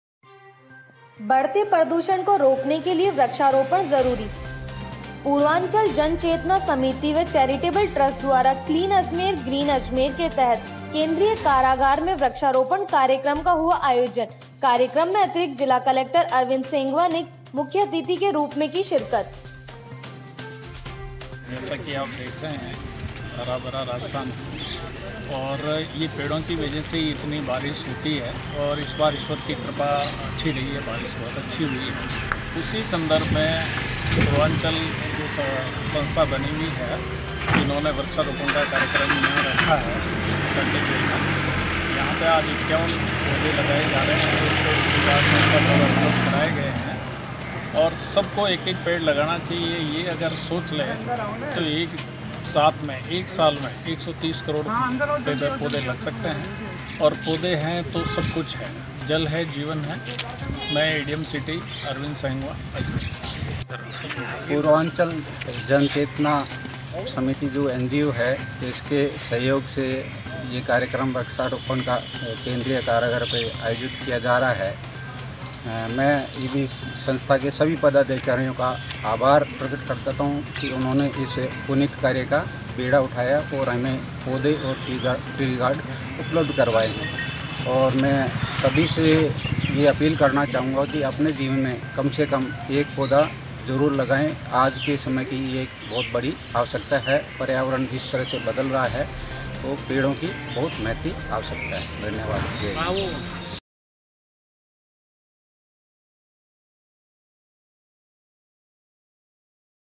बढ़ते प्रदूषण को रोकने के लिए वृक्षारोपण जरूरी
पूर्वांचल जन चेतना समिति व चैरिटेबल ट्रस्ट द्वारा क्लीन अजमेर व ग्रीन अजमेर के तहत केंद्रीय कारागार में वृक्षारोपण कार्यक्रम का हुआ आयोजन कार्यक्रम में अतिरिक्त जिला कलेक्टर अरविंद सिंह ने मुख्य अतिथि के रुप में की शिरकत